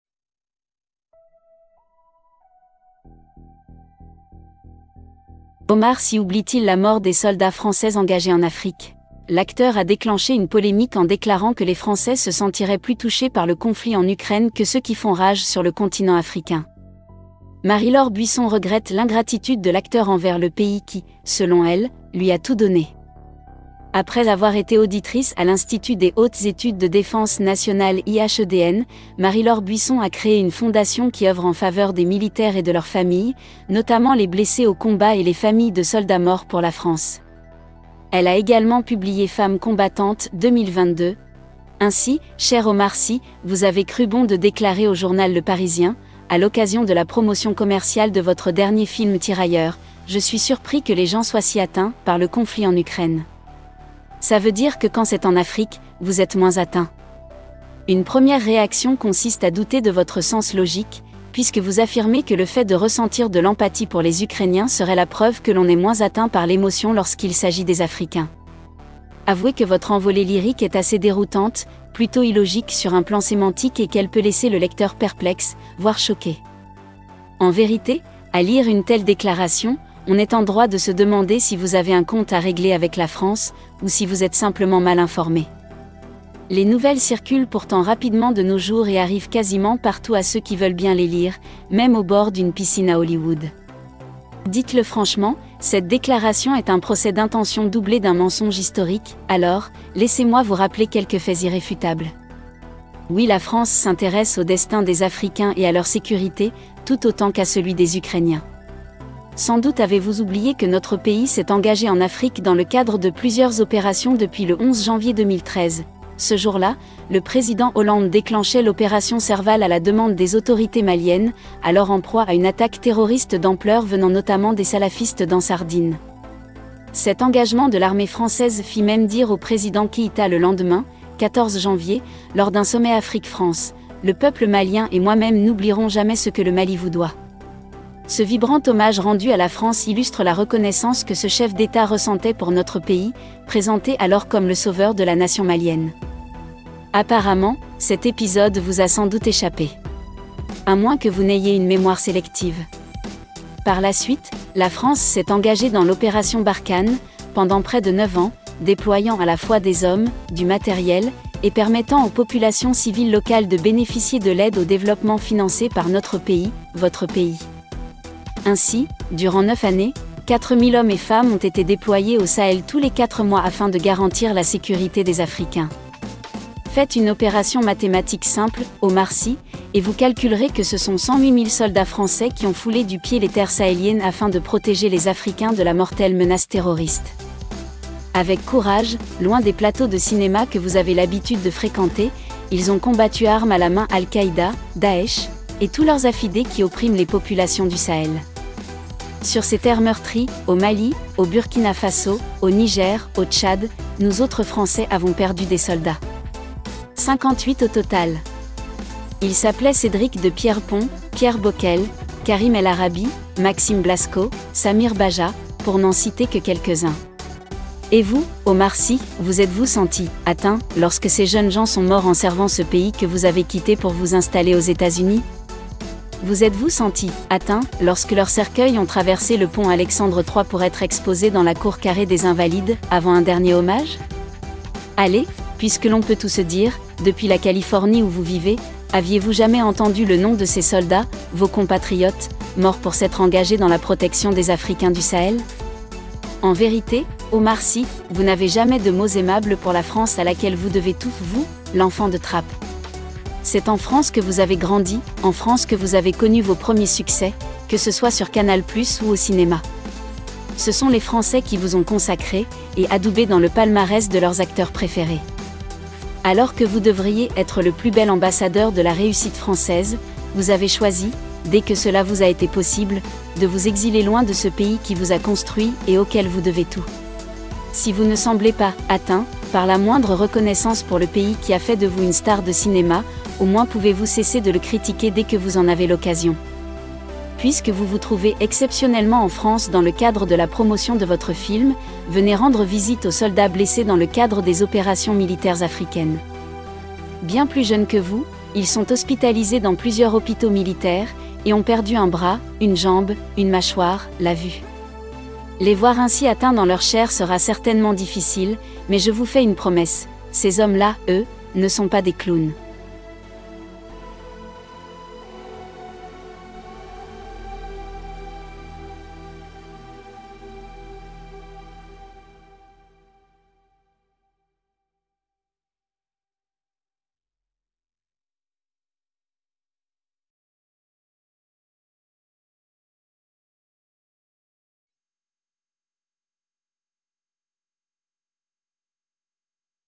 OMAR SY OUBLIE LES SOLDATS FRANÇAIS TOMBÉS EN AFRIQUE
Omar Sy provoque la polémique pendant la promotion de son dernier film "Tirailleurs". Il reproche a la France son absence d'engagement pour l'Afrique contrairement a celui pour l'Ukraine. Oubliant ou ignorant que plusieurs dizaines de soldats français sont tombés en Afrique dans la dernière décennie uniquement.